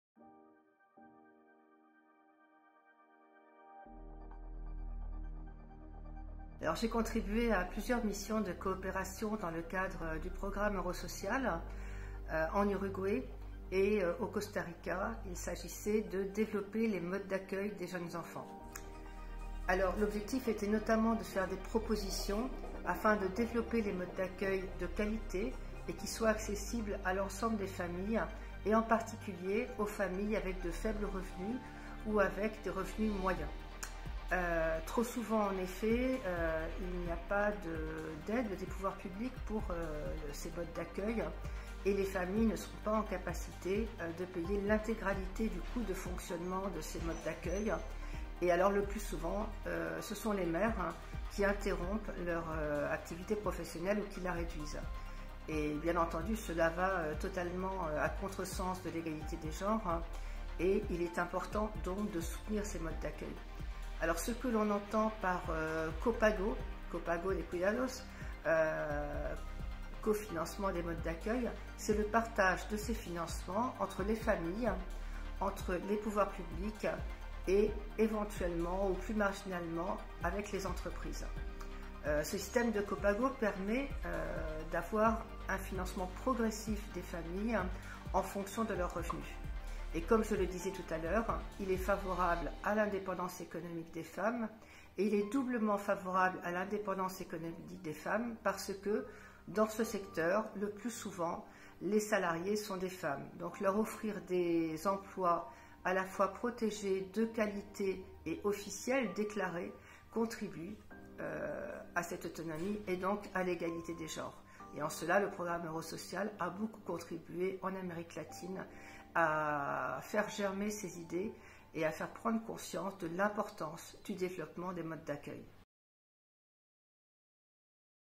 Interview Frédérique Leprince, experte mobilisée sur le programme Eurosocial+
Frédérique Leprince, experte mobilisée sur le programme Eurosocial+, nous explique les enjeux de la prise en compte des questions d'égalité de genre dans les services d'accueil et de développement de l'enfant.